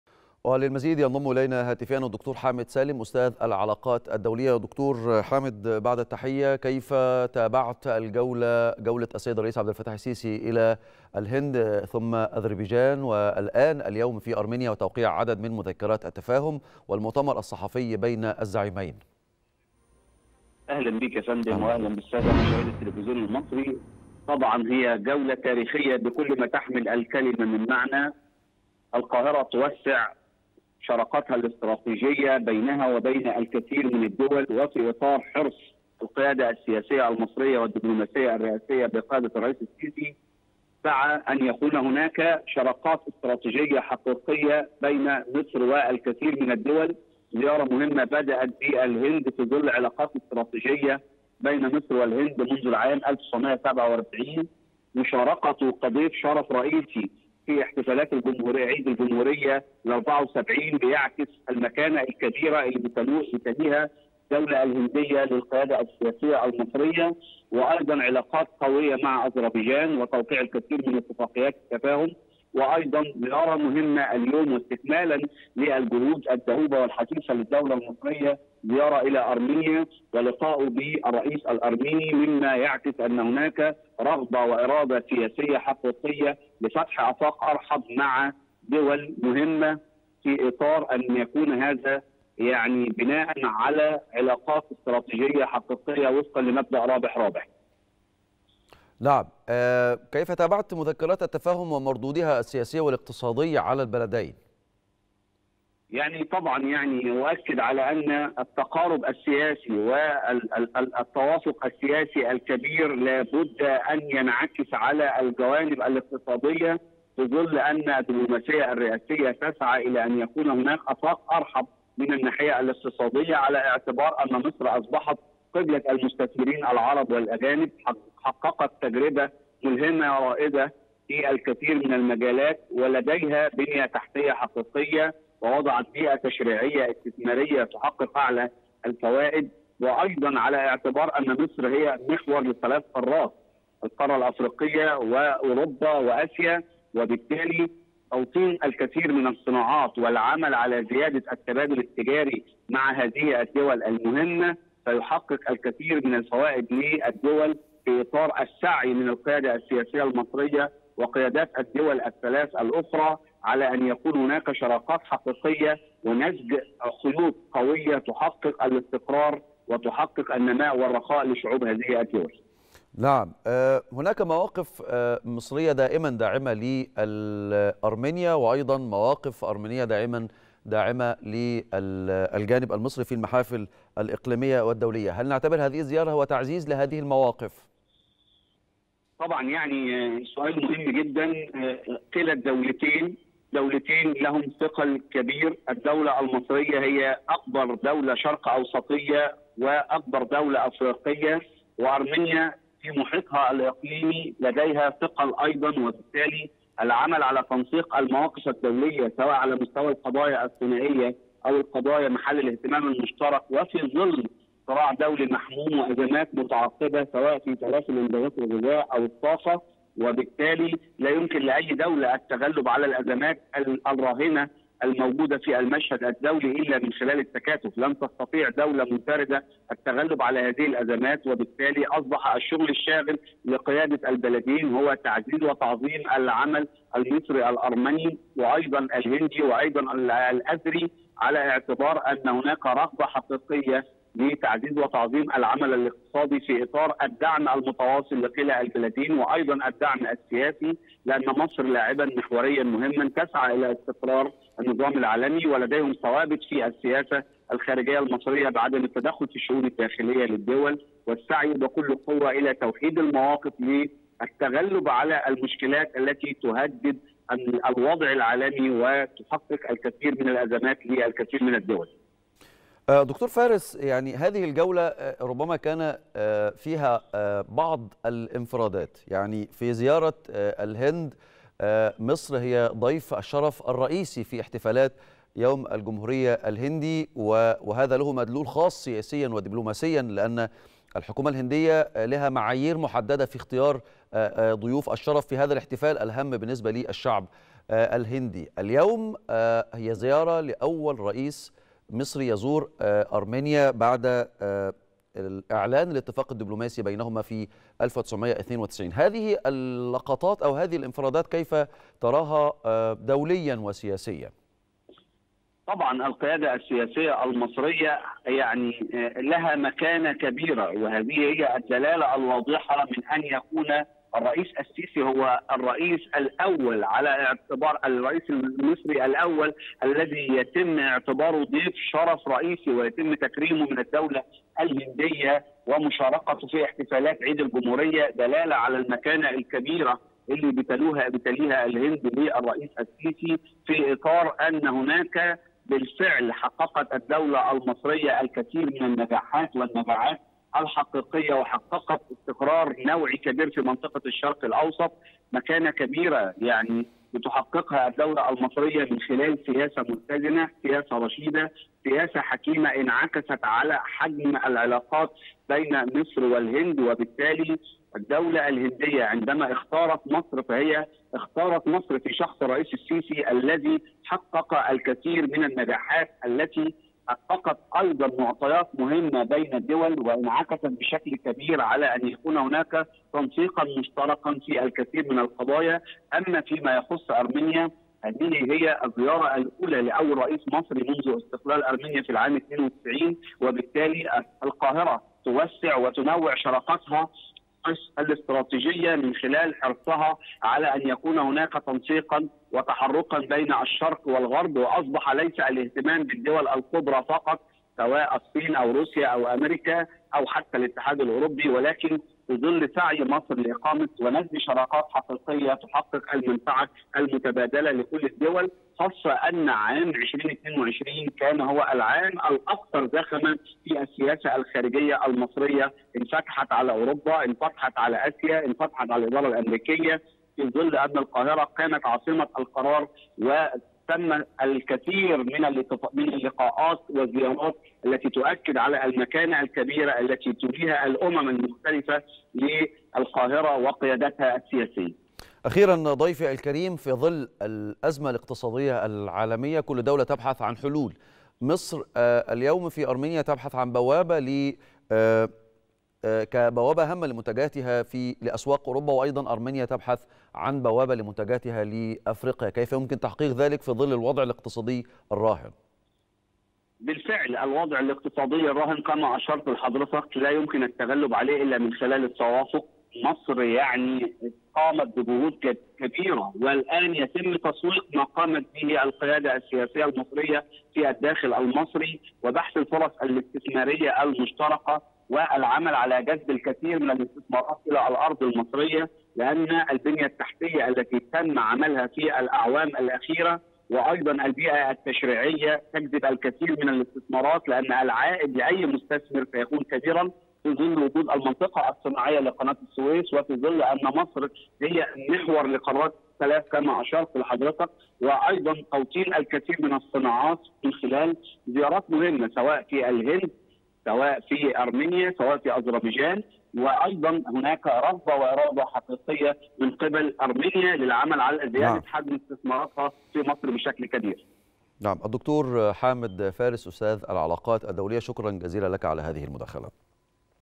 د. حامد فارس يوضح أهمية زيارة الرئيس السيسي إلى أرمينيا في تكوين علاقات استراتيجية قوية
تغطية خاصة | د. حامد فارس يوضح أهمية زيارة الرئيس السيسي إلى أرمينيا على رغبة القيادة السياسة في تكوين علاقات استراتيجية قوية